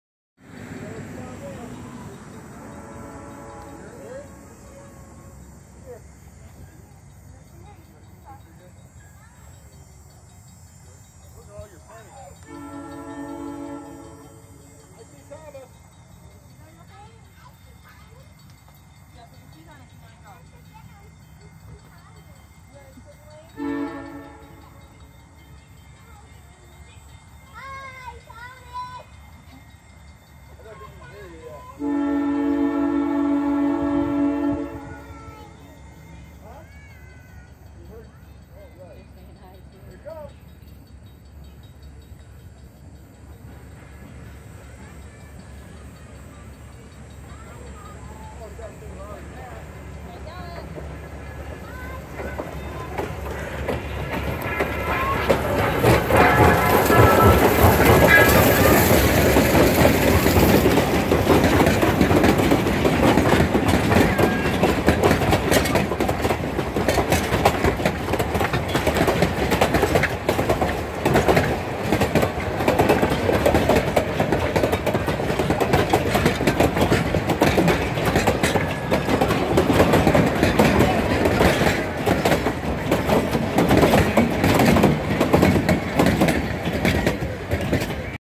Crossroads Village & Huckleberry Railroad Day out with Thomas Last Run of the day
Thomas The Tank Engine Number 1
Huckleberry Railroad Number 7